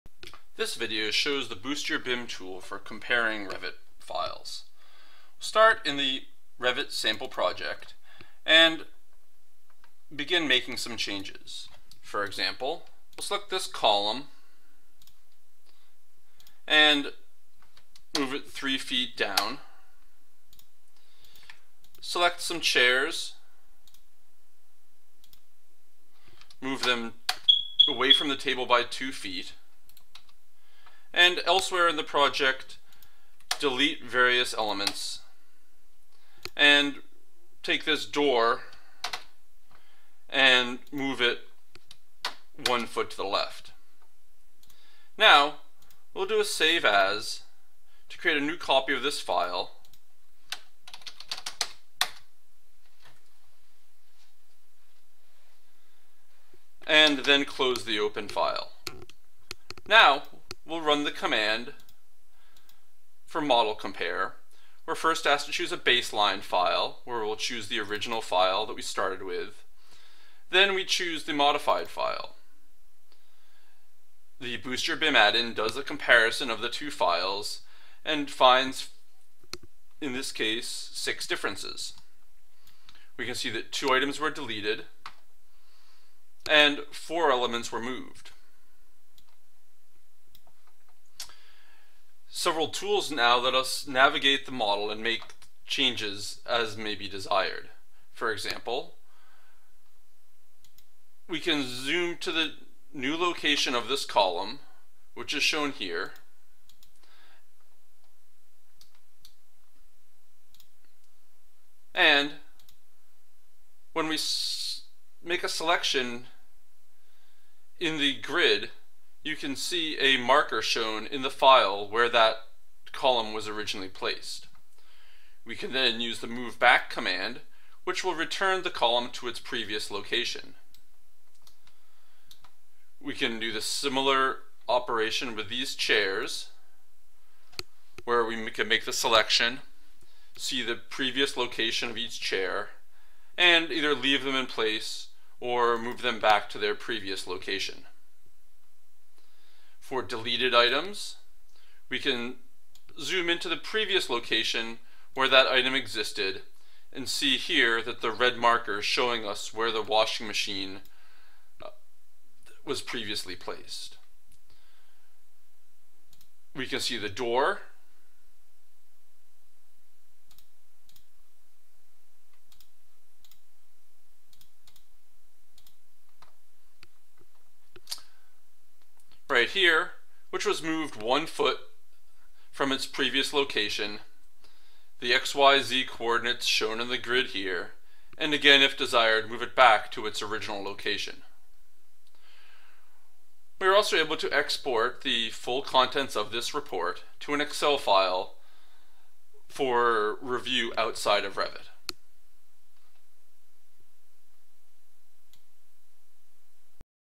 Boost Your BIM Revit Model Compare
Compare two versions of a Revit file
See the previous location of moved elements, and optionally move them back to their previous location